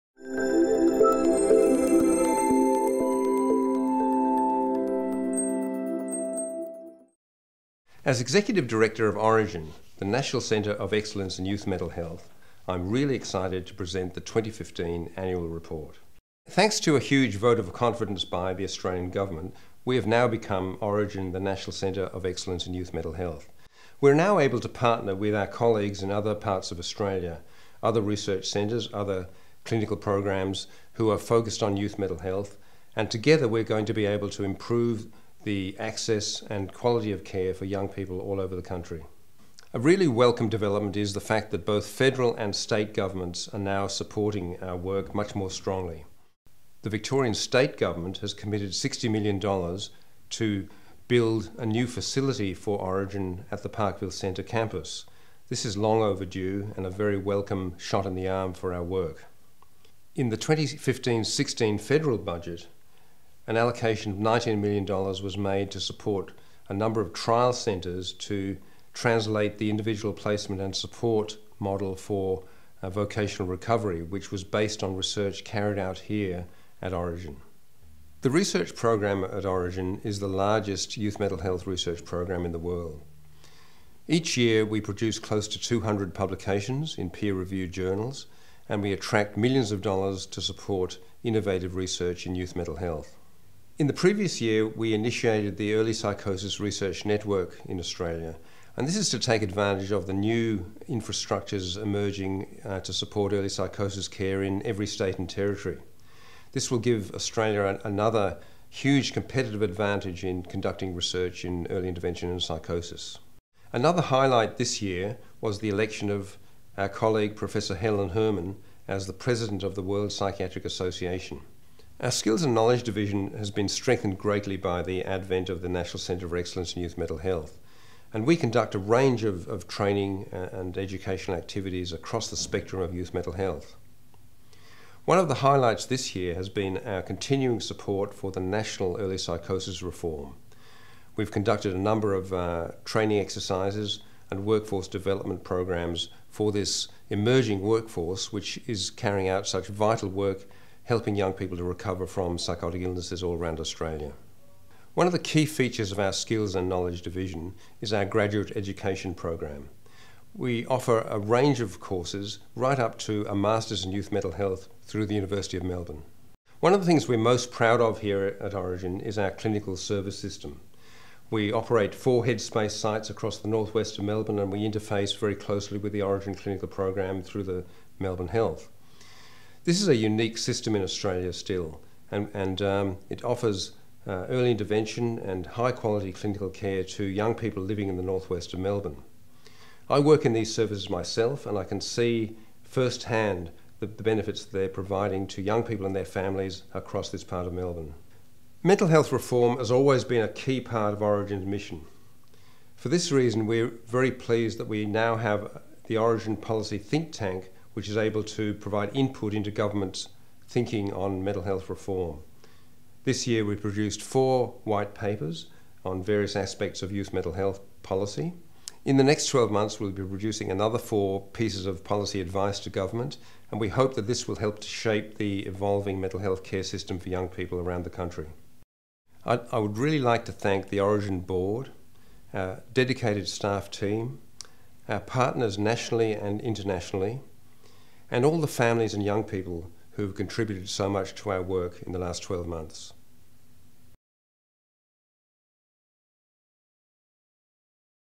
Orygen Annual Report 2015
We've just released our Annual Report for 2015, the first as The National Centre of Excellence in Youth Mental Health. Watch the video of our Executive Director, Professor Patrick McGorry AO reflecting on the year that was.